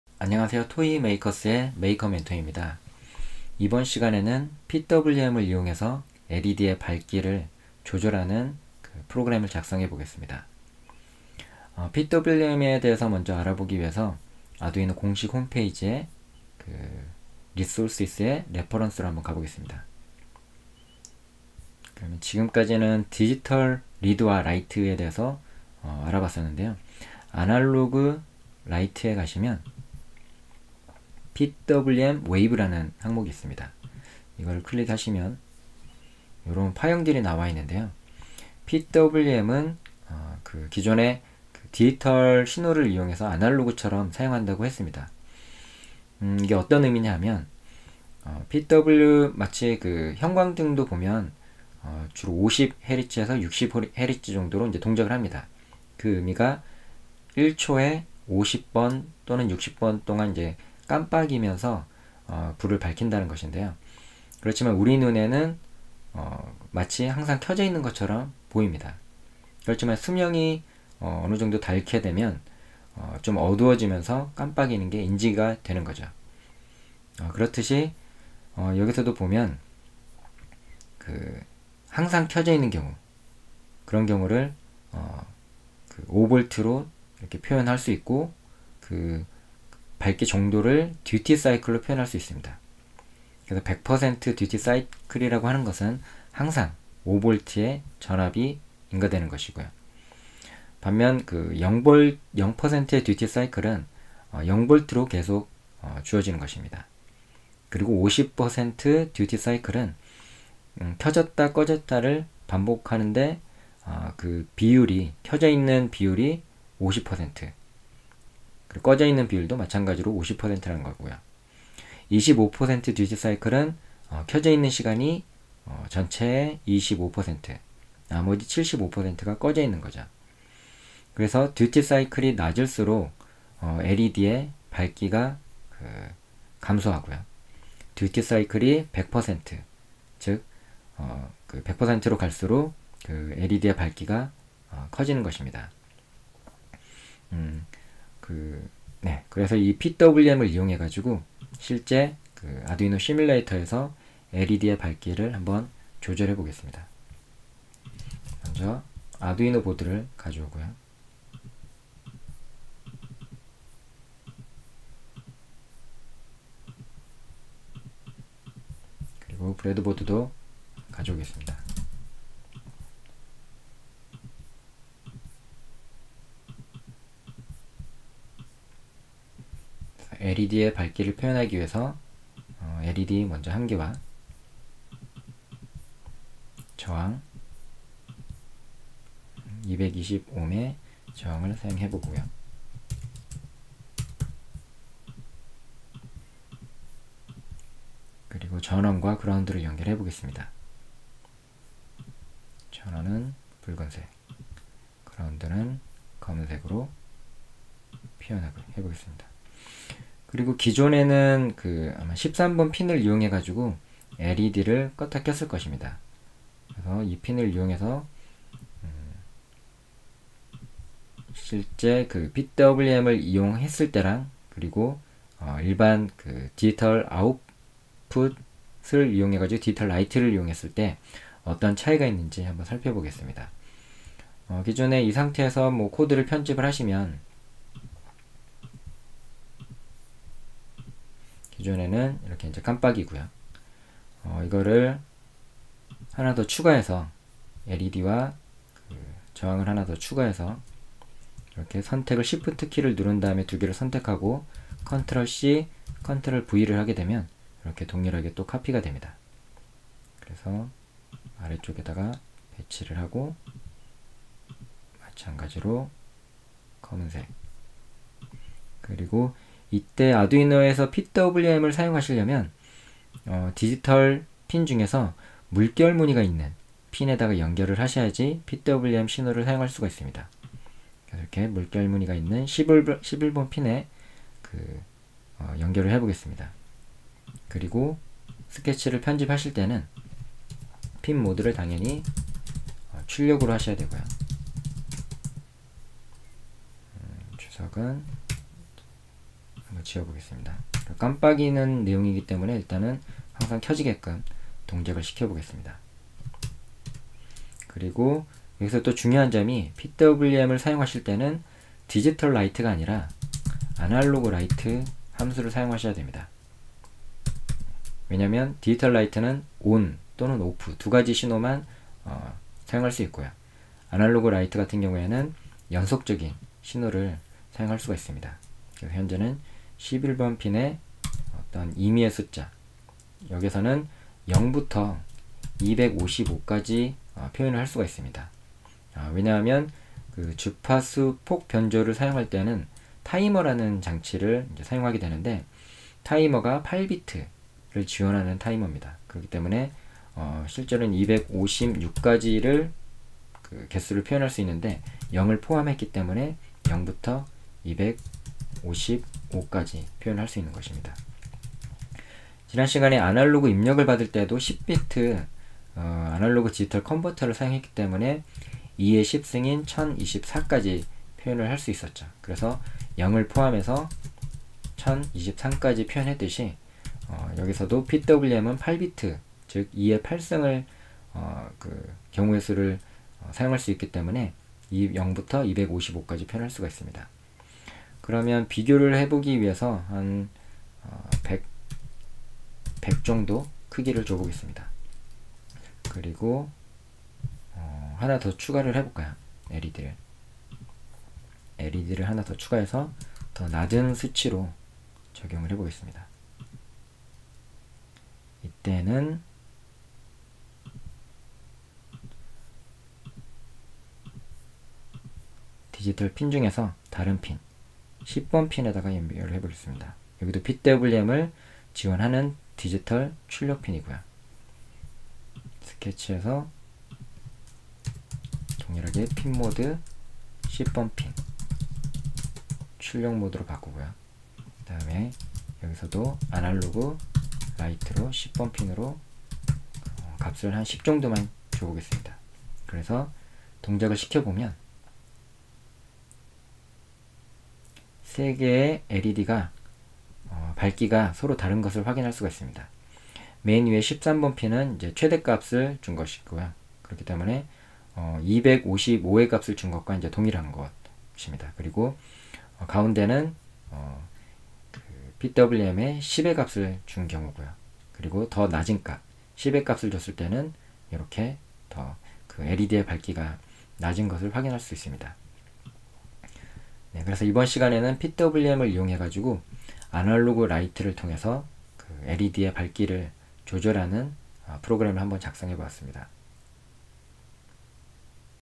아두이노 시뮬레이터 사용하기 11편 - LED 밝기 조절하기 [자막 포함] (ENG SUB)
아두이노 시뮬레이터 에서 PWM 을 이용해 LED 의 밝기 를 조절하는 프로그램을 작성해보겠습니다.

먼저 PWM에 대해 알아보기 위해 아두이노 공식 홈페이지 [RESOURCES] ㅡ)[REFERENCE] 메뉴를 선택합니다.
analogWrite 함수에는 PWM wave 항목이 있습니다. 이를 클릭하면 여러 파형들을 볼 수 있습니다.
PWM은 기존의 디지털 신호를 이용해 아날로그처럼 사용하는 것입니다.